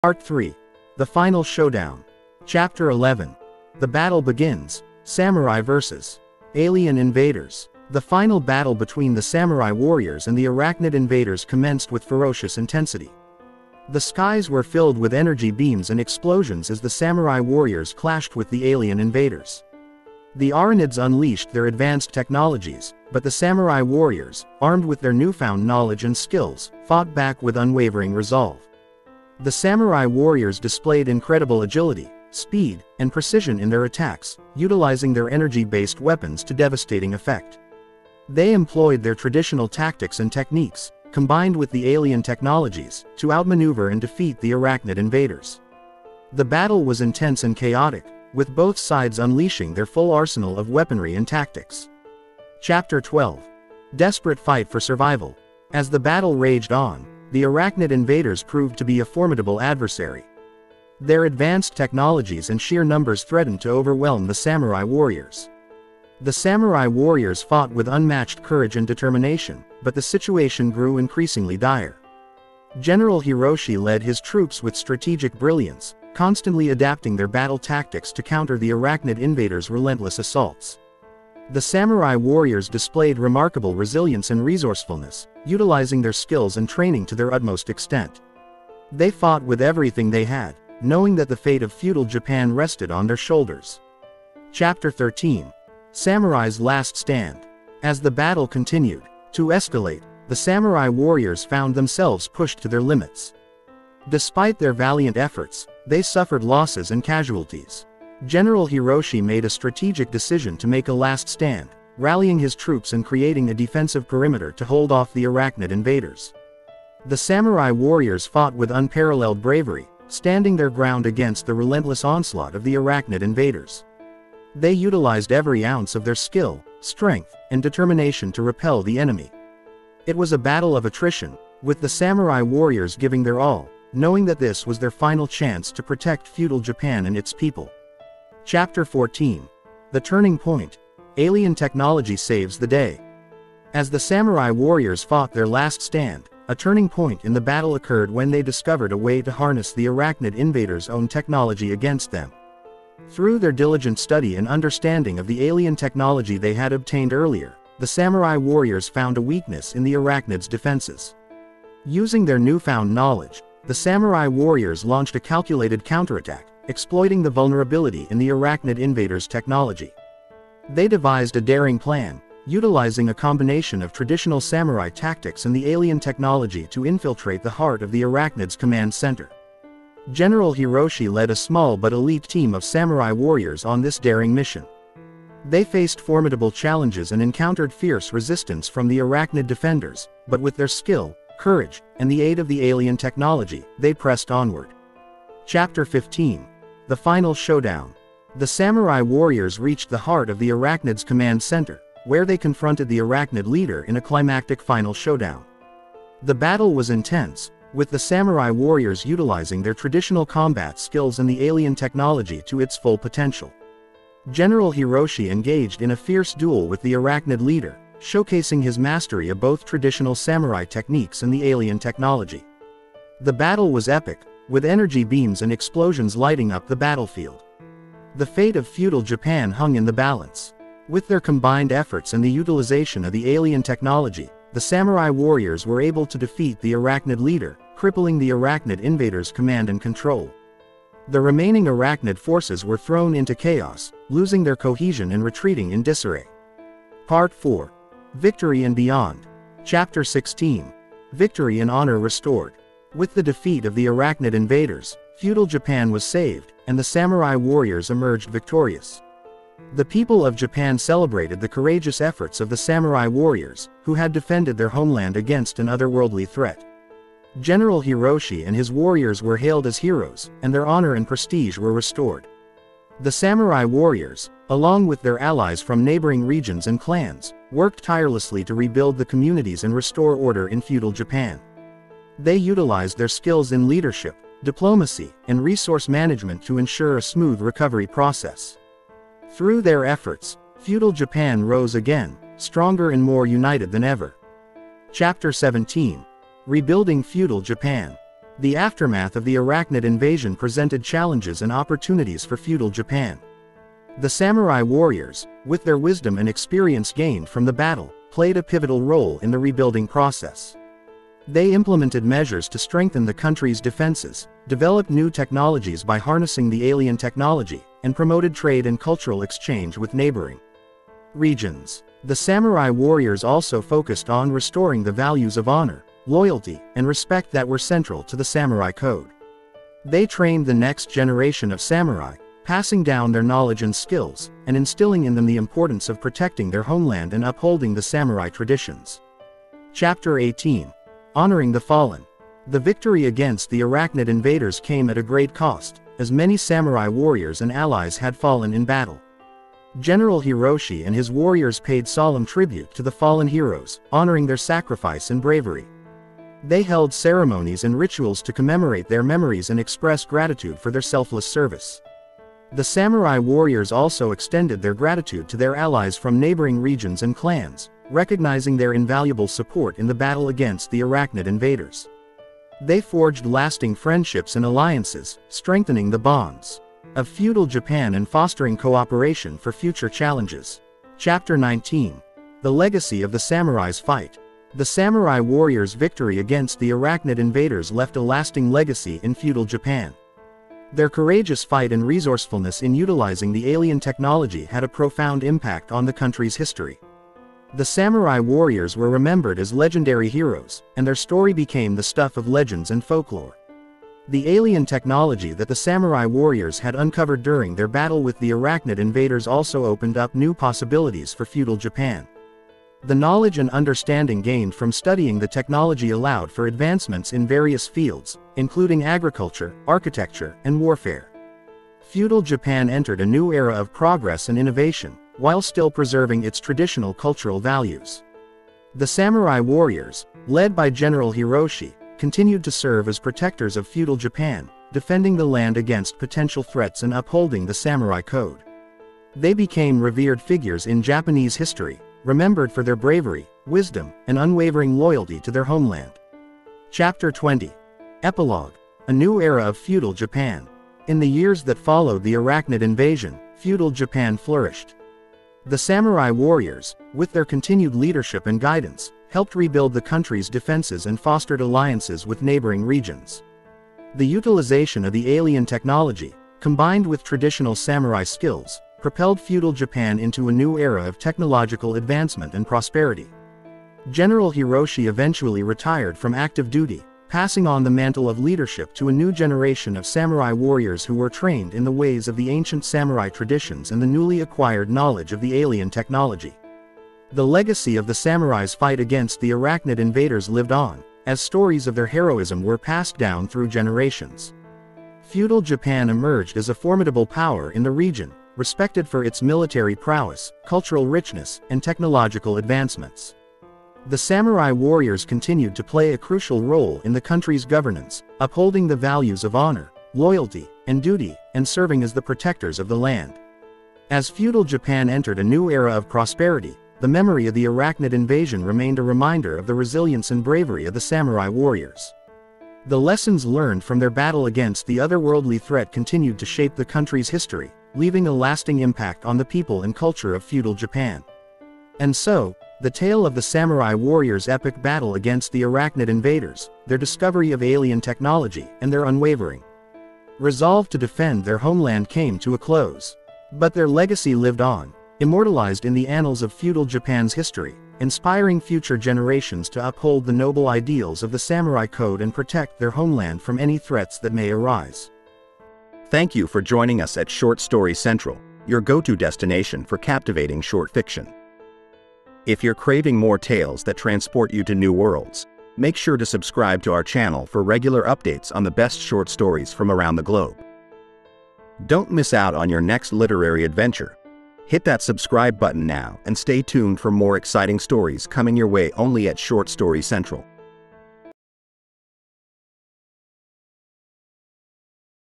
Warriors from Beyond: Samurai's Fight for Honor - Part 2
In Feudal Japan, a group of samurai warriors stumble upon a crashed alien spaceship in the mountains. Inside, they discover advanced technologies that they have never seen before. With the help of a local scientist, they learn how to use alien weapons and devices to defend their land against a looming threat from beyond the stars.

As they train and prepare for battle, they discover that the alien ship was not the only one to crash on Earth. A group of hostile aliens have been watching the planet for centuries, waiting for the right moment to invade. With the samurai warriors as their only hope, they must use their newfound knowledge and skills to stop the alien invasion and save Feudal Japan.

As the battle rages on, the samurai warriors face impossible odds and must rely on their courage, strength, and honor to protect their people. With the help of alien technologies, they are able to turn the tide of the war and emerge victorious.